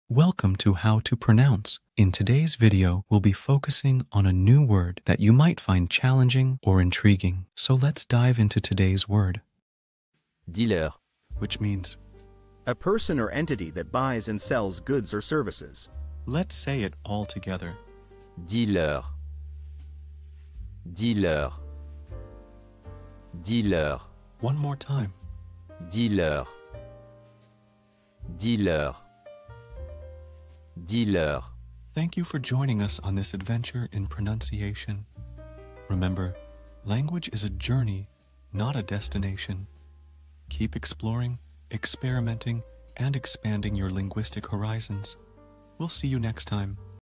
How to pronounce Dealer
🌍 Learn a new language now Master the Pronunciation of 'Dealer which means Dealer' - which means : A person or entity that buys and sells goods or services. 📝 with @HTPronounce 🎓🗣️ - [Your Guide to French]

Learn to pronounce 'Dealer' perfectly in French with our easy and engaging tutorial by @HTPronounce! 📖📽️ In this comprehensive video, we provide you with native speaker examples 🗣️👂 and phonetic breakdowns 🎵 to ensure you get the pronunciation just right.

How to say Dealer in French